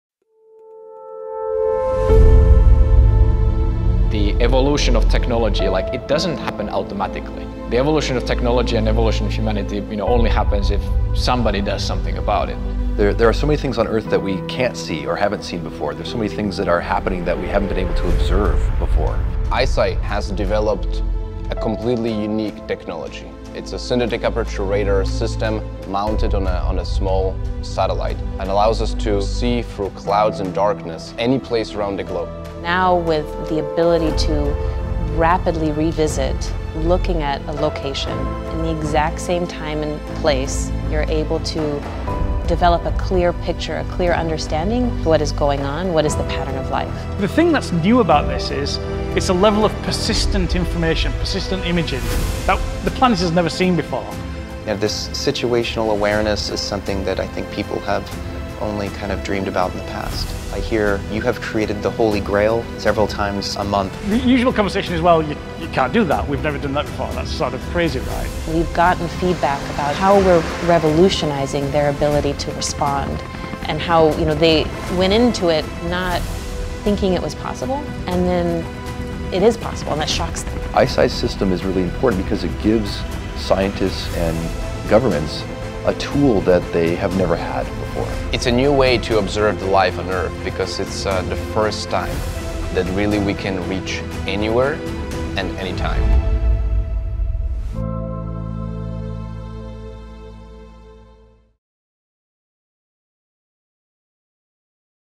It's A New Way To Understand Earth | ICEYE
ICEYE's new space SAR satellites give you valuable data on any location on Earth – day and night, in any weather, multiple times a day.

There are so many things happening on Earth that we have never been able to observe, until now. ICEYE’s Persistent Monitoring capability uses Synthetic Aperture Radar (SAR) to deliver actionable intelligence in near real-time. It’s a global capability that’s powering governments and commercial organizations to detect and respond to changes on Earth in ways they’ve only dreamed about.